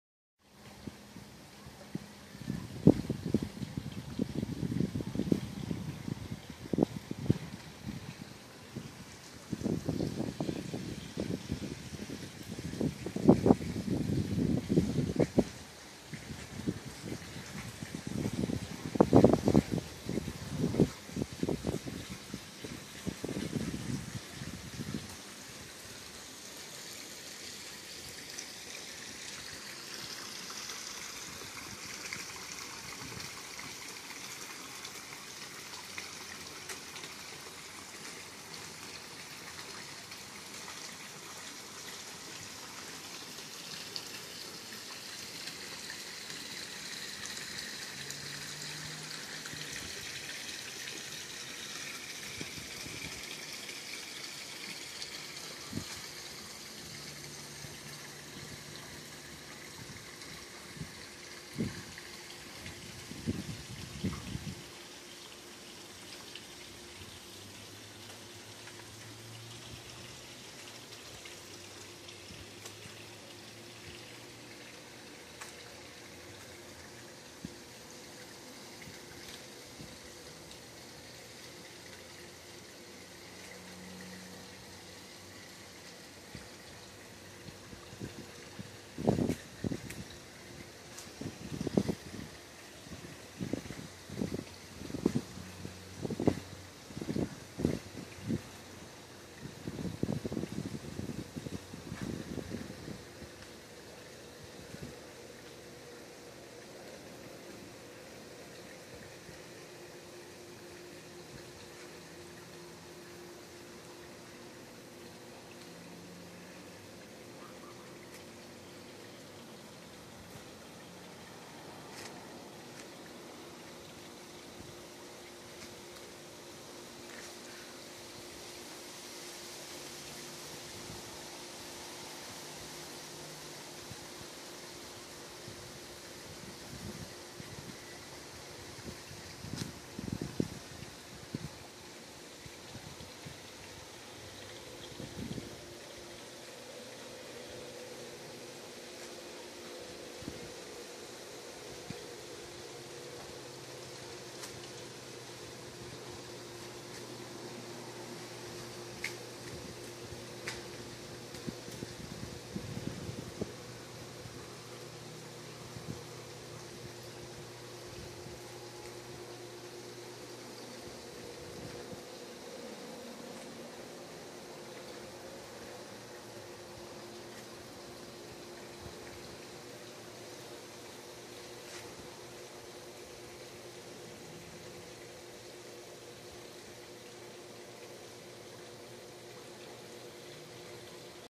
Breathtaking Waterfront @ Blue Pelican
The pool deck of the Blue Pelican is breathtaking and offers a one-of-a-kind spa like indulgence in relaxation and luxury.